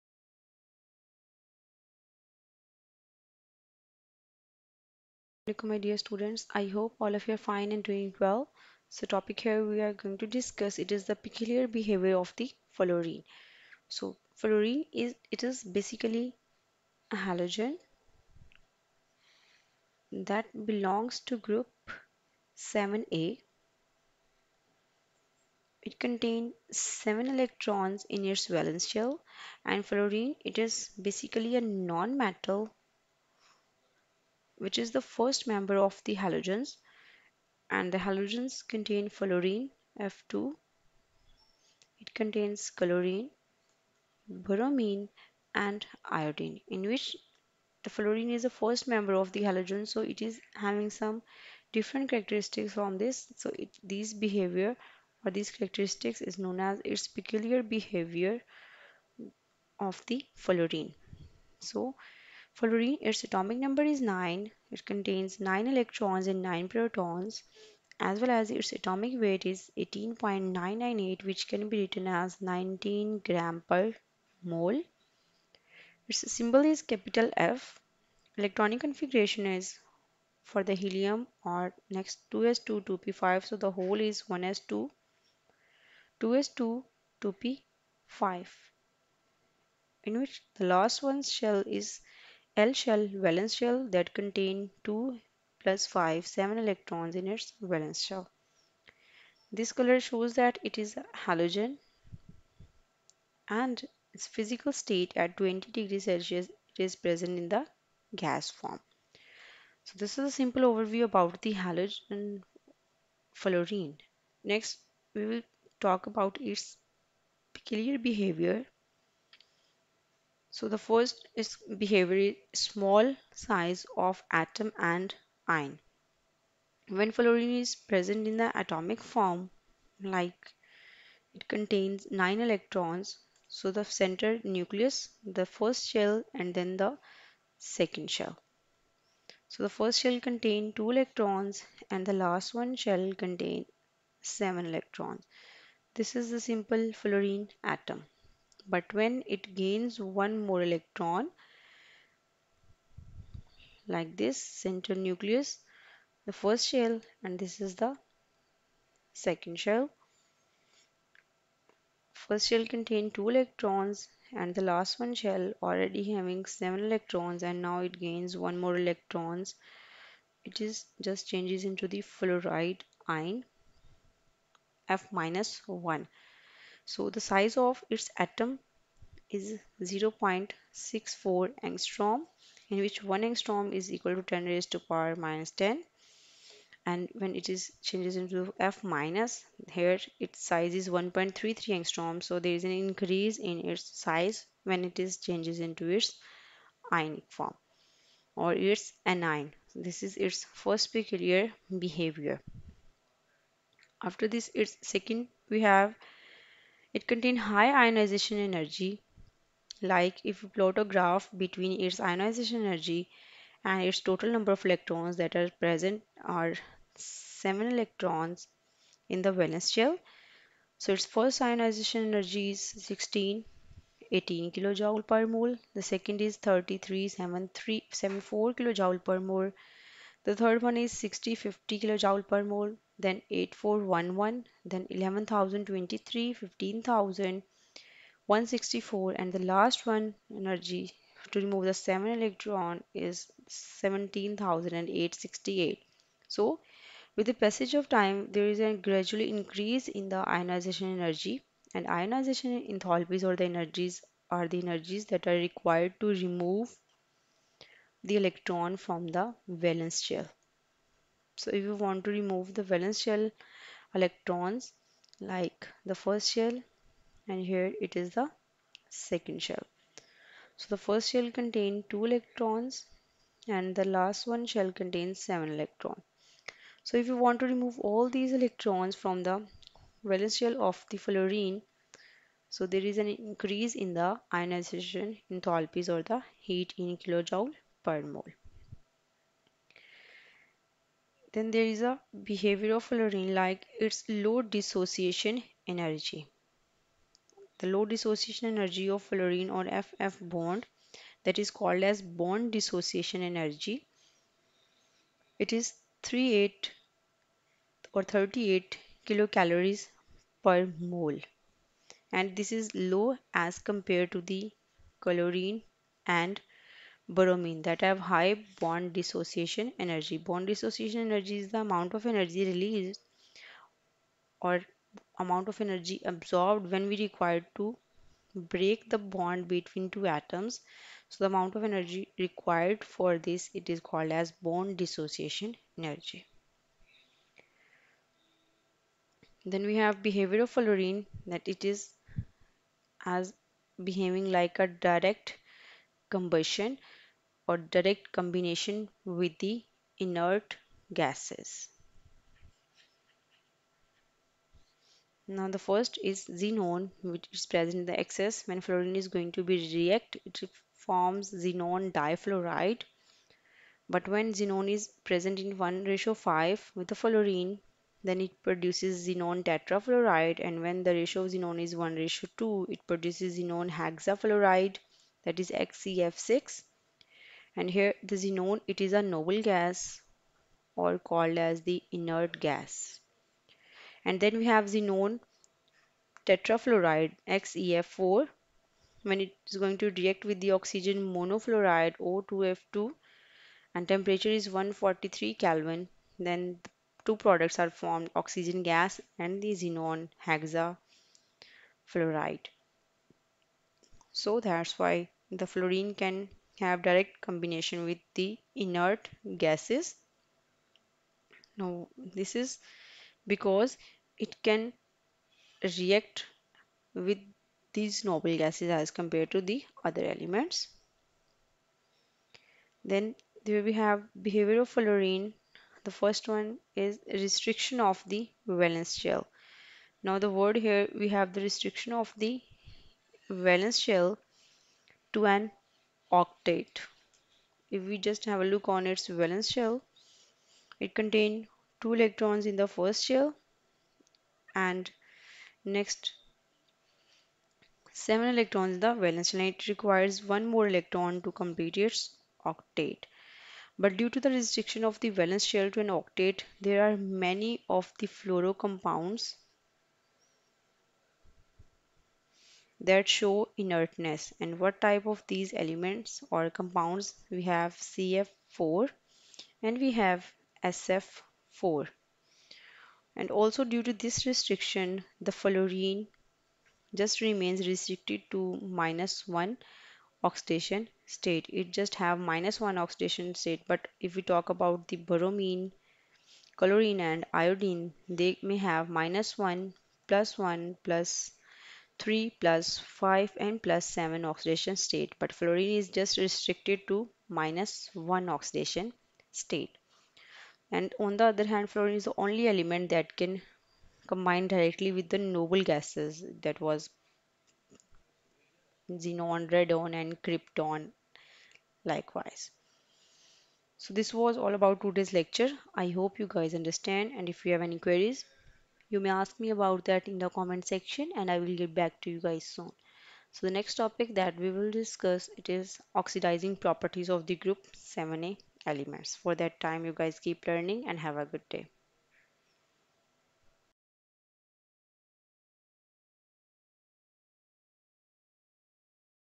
Peculiar Behaviour of Fluorine: Unraveling the Reactive Nature | Grade 12 Chemistry | Lecture 3
Explore the unique behavior of fluorine, an exceptionally reactive element in the halogen family. This Grade 12 Chemistry lecture delves into the distinctive properties and reactions that define fluorine's chemical personality. Gain a deeper understanding of how fluorine's reactivity plays a pivotal role in various chemical processes and industrial applications.